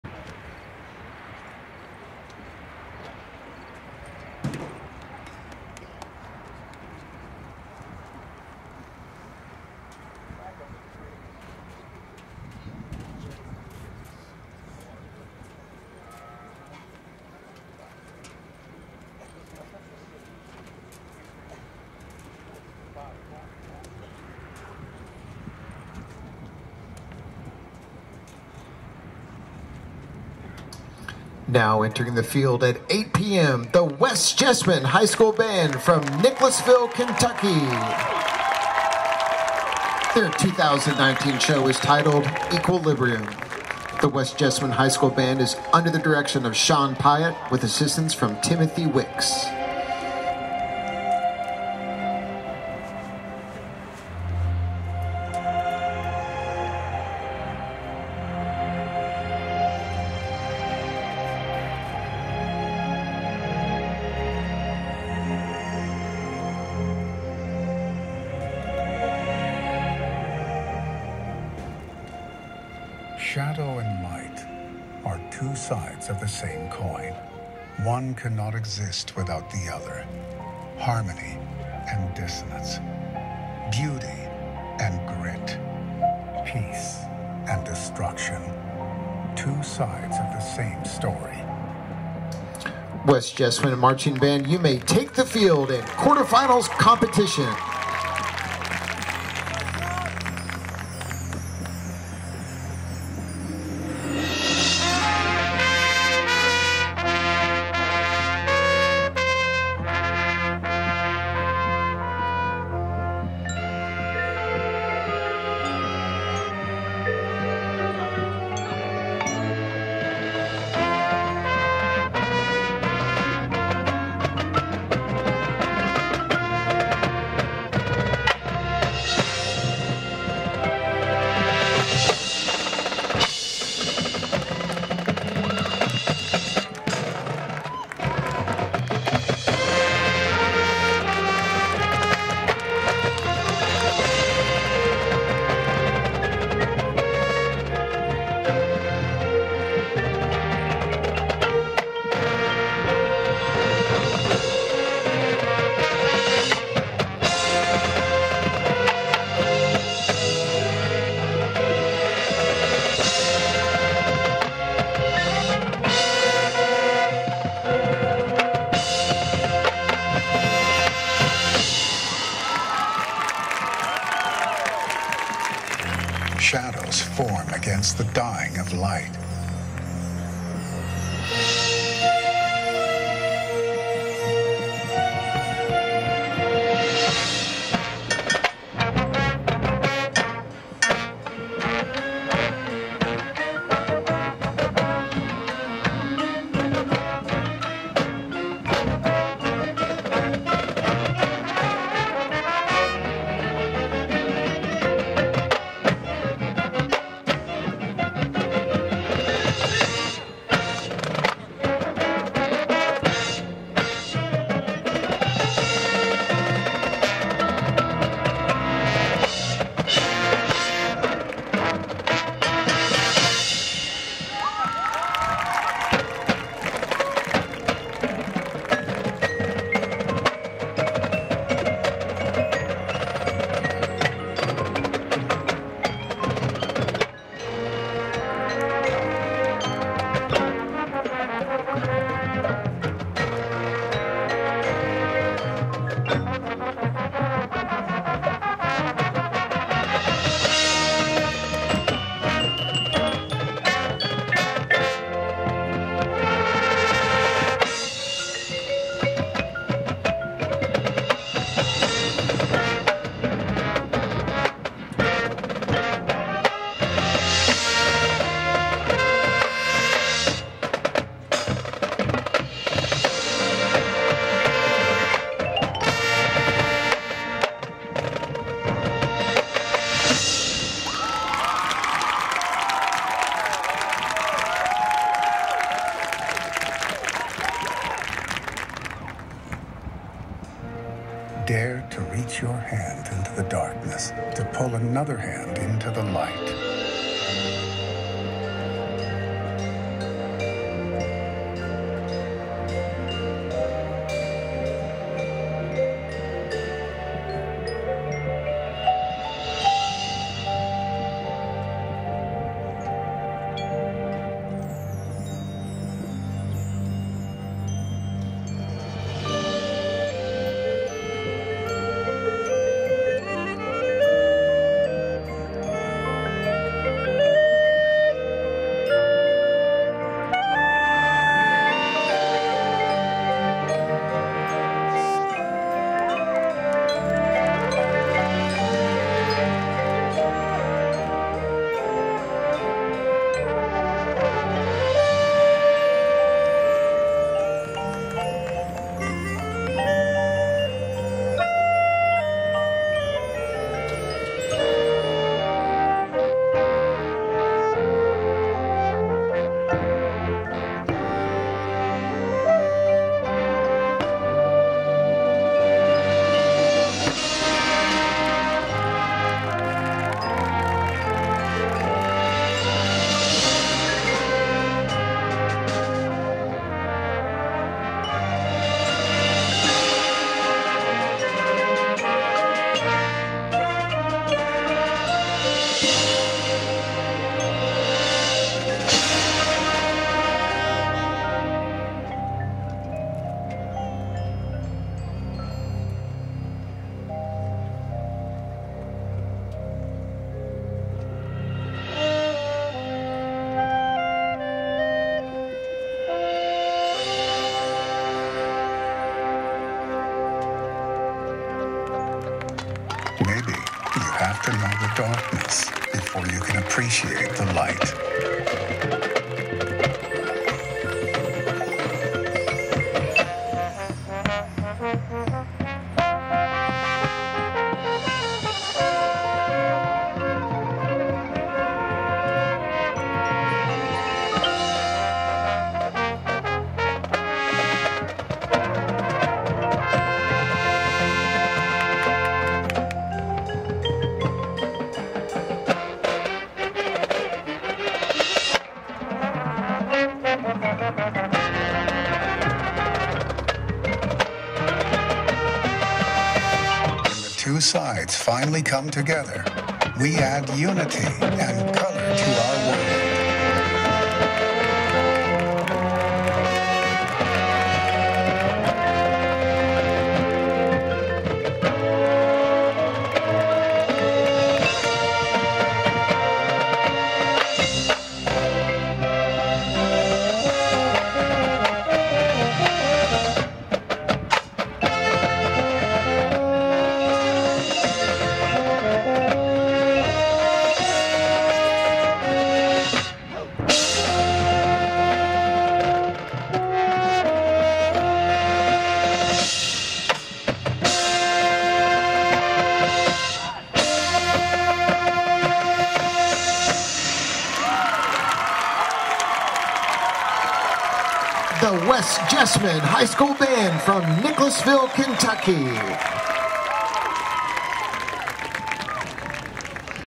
West Jessamine Marching Band - 2019 KMEA Quarterfinals
Class - AAA East
Score - 74.09
Placement - 8th overall
Location - George Rogers Clark High School